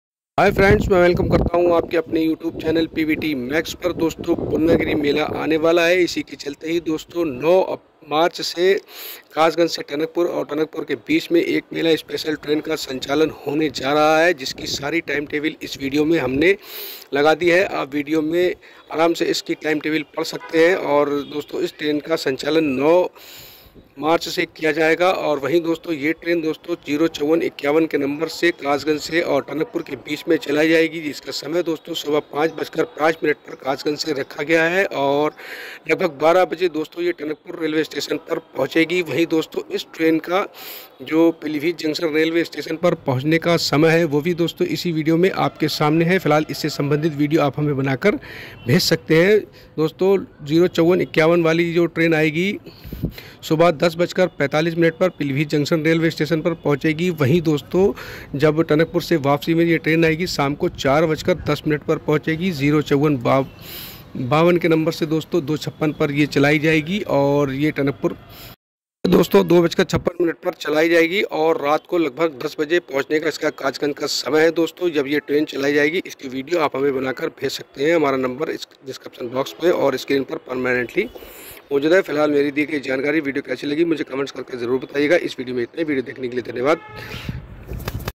टनकपुर-पीलीभीत को मिली 1 नई ट्रेन की सौगात,, अब चलेगी टनकपुर से कासगंज की नई ट्रेन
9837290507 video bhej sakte